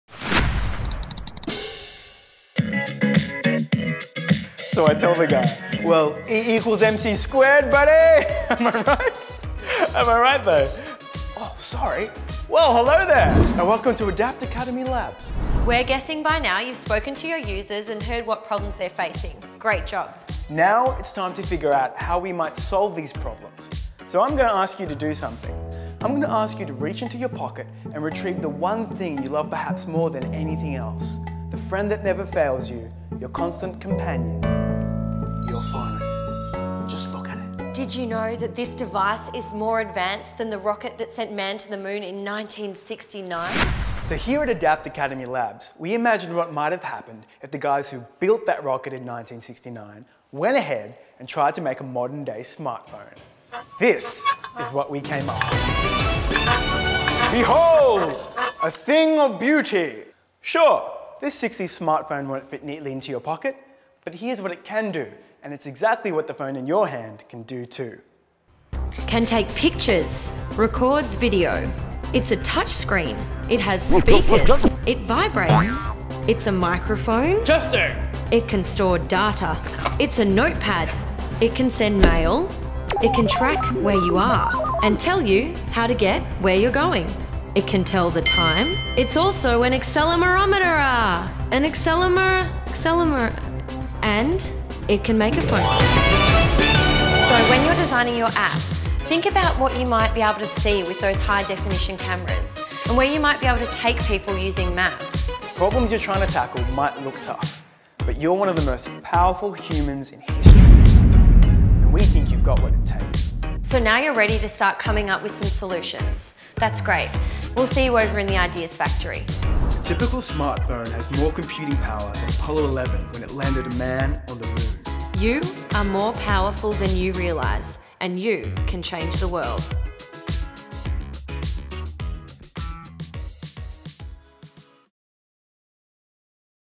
ADAPPT LESSON TWO: The Modern Telephone
The average smartphone has more computing power than the Apollo 11 rocket which sent a man to the moon. We take stock of all the functions at your fingertips.